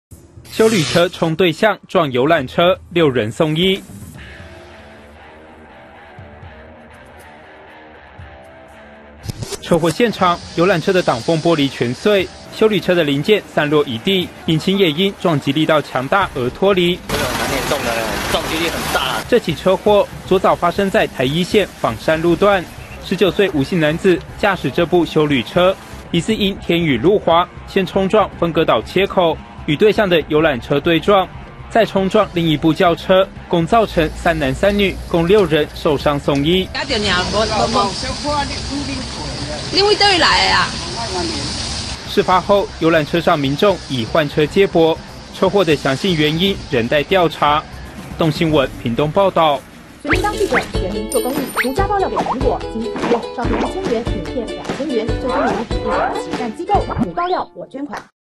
休旅車衝對向撞遊覽車 6人送醫--蘋果日報20150525
【動新聞╱屏東報導】屏東昨大雨不斷，一輛黑色休旅車行經台一線枋山鄉南下路段時，疑天雨路滑，加上車速過快，猛烈撞上分隔島切口，波及到同向的一輛銀色轎車，與北上的一輛遊覽車，造成6人受傷送醫，其中黑色休旅車的女性乘客傷勢嚴重，意識不清但生命徵象穩定，仍在加護病房觀察中。車禍現場散落一地的車體零件碎片，怵目驚心。
警方調查，昨上午9時許黑色休旅車駕駛吳廷彰（19歲）、搭載陳品儒（18歲）要南下墾丁，遊覽車由黃朝河（41歲）所駕駛，乘載彰化員林的長春俱樂部到台東遊玩，準備北返彰化，發生事故後，車上40名人員改由他車接駁。但突如其來的意外，也讓老年人大呼：「都快嚇昏了！」

休旅車衝對向撞遊覽車
6人送醫
車禍現場
遊覽車的擋風玻璃全碎
休旅車的零件散落一地
引擎也因撞擊力道強大而脫離

真的滿嚴重的
撞擊力很大啦

這起車禍
昨早發生在台一線枋山路段
19歲吳姓男子駕駛這部休旅車
疑似因天雨路滑
先衝撞分隔島切口
與對向的遊覽車對撞
再撞擊另一部轎車
共造成3男3女
共6人受傷送醫

有沒有嚇到
有一點撞破皮
你從哪裡來
彰化員林

事發後
遊覽車上民眾已換車接駁
車禍的詳細原因仍待調查
動新聞 屏東報導